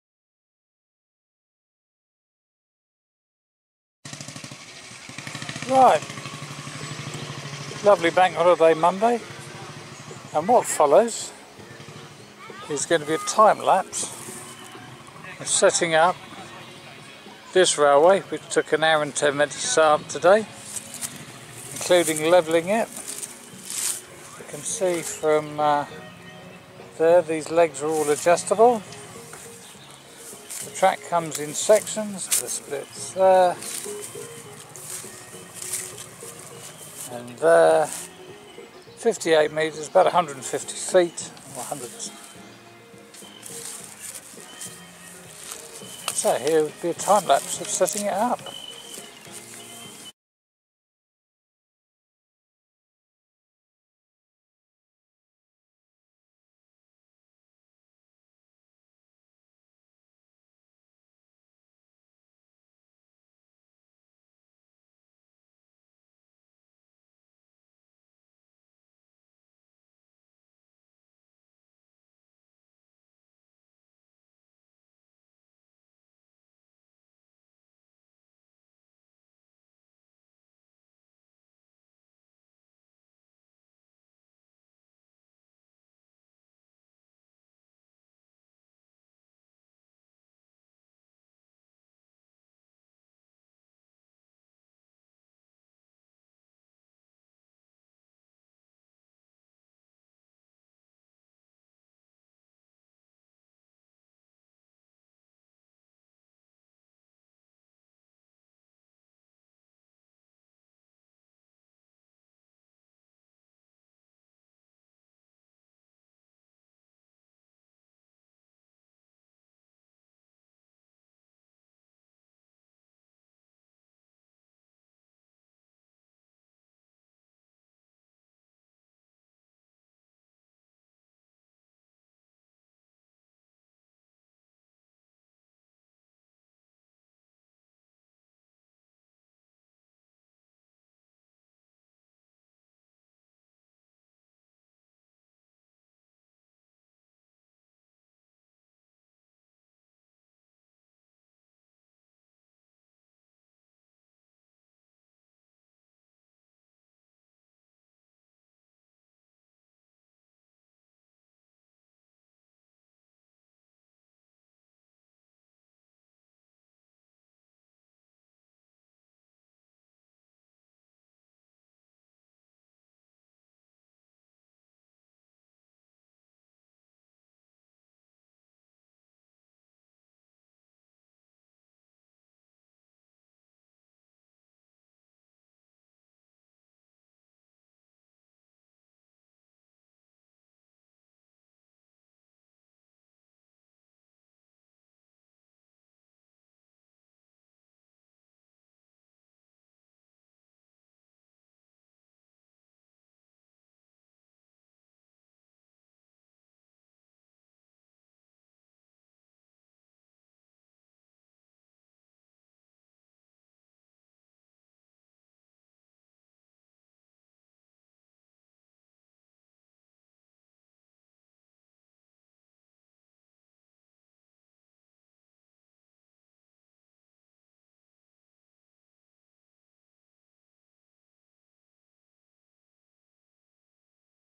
Setting up a portable miniature railway in time lapse
Here is a video of setting up a 5" gauge model railway to give rides to the public.